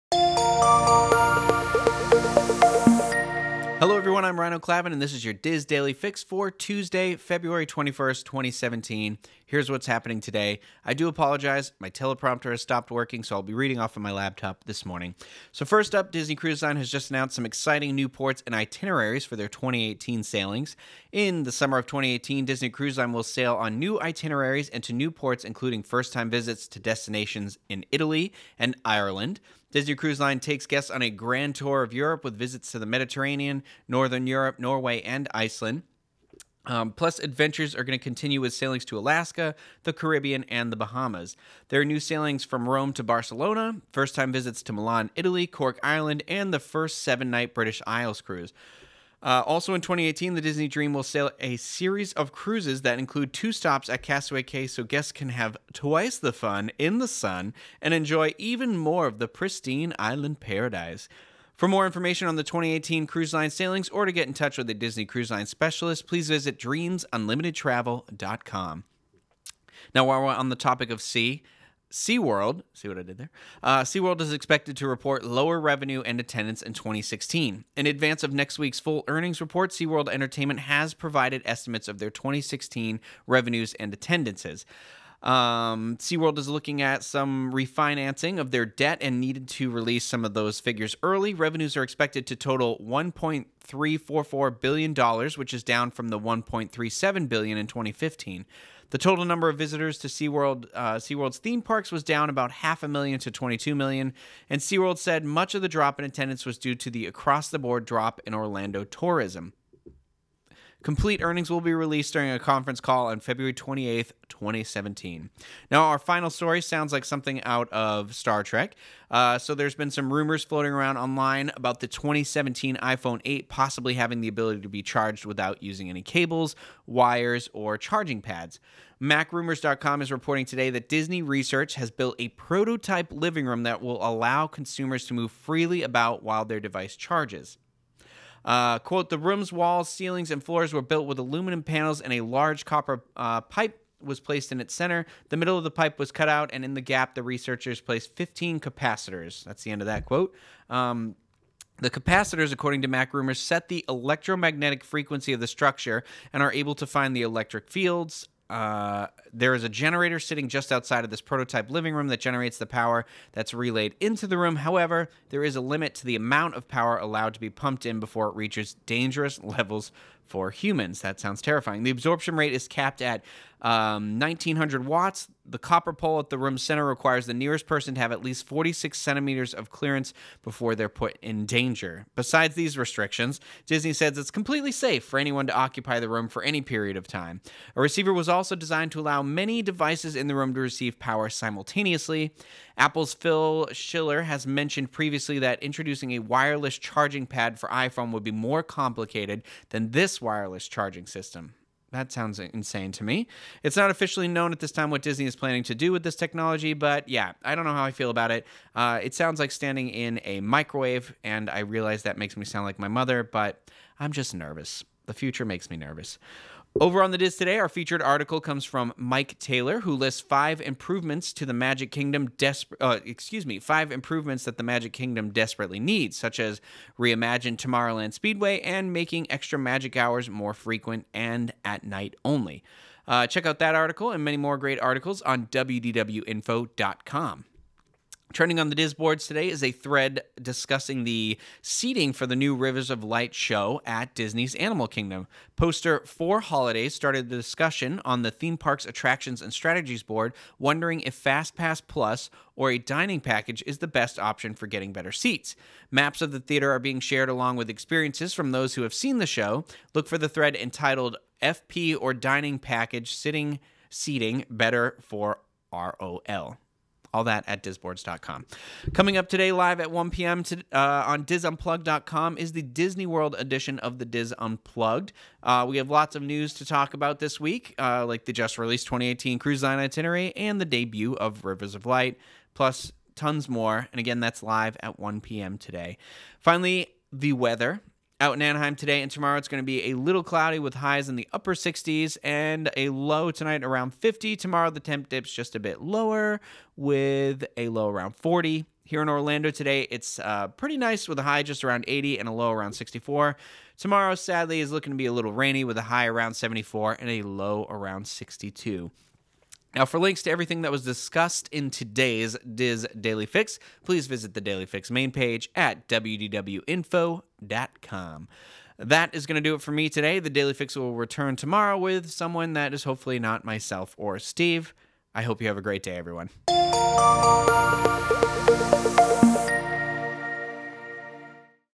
DIS Daily Fix | Your Disney News for 02/21/17
Disney Cruise Line releases 2018 itineraries, SeaWorld struggles, and more in today's DIS Daily Fix!